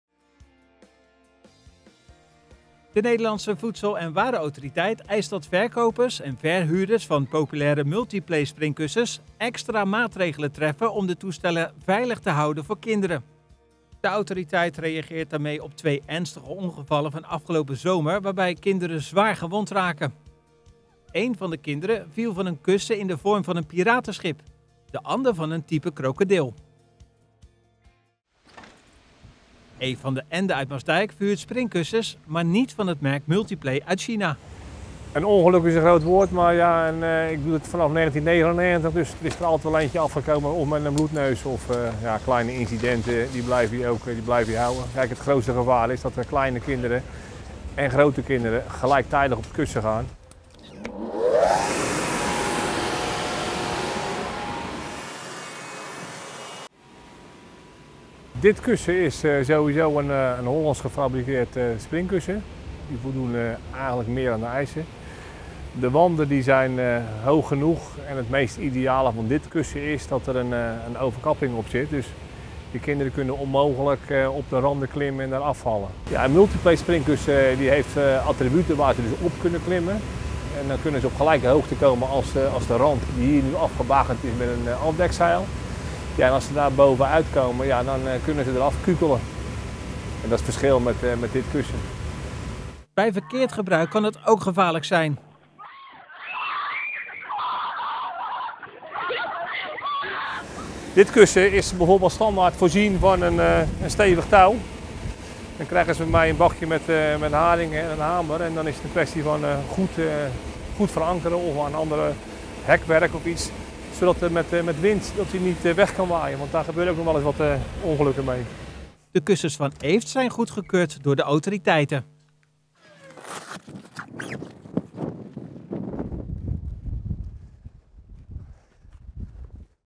Maatregelen tegen onveilige springkussens
De Nederlandse Voedsel- en Warenautoriteit eist dat verkopers en verhuurders van populaire Multiplay-springkussens extra maatregelen treffen om de toestellen veilig te houden voor kinderen.